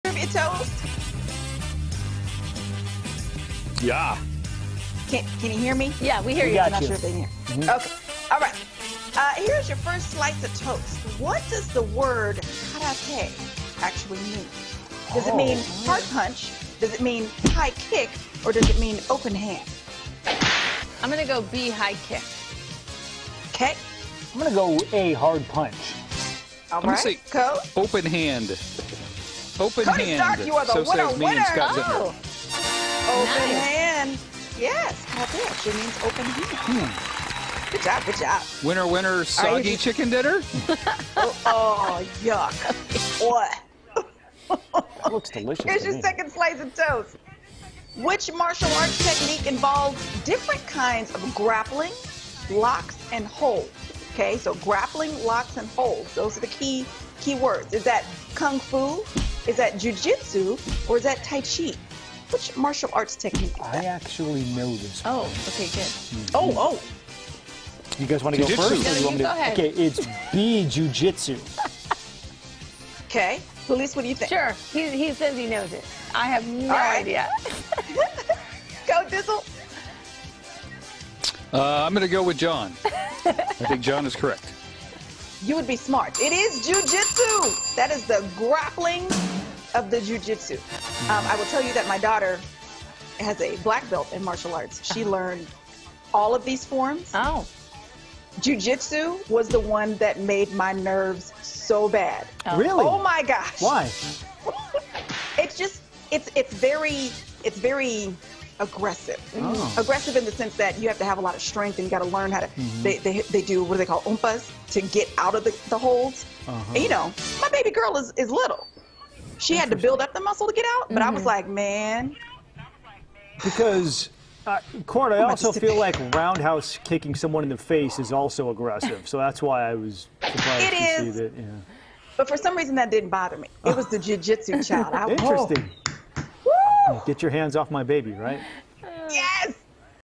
Trivia Toast
Court is testing the hosts knowledge on martial arts.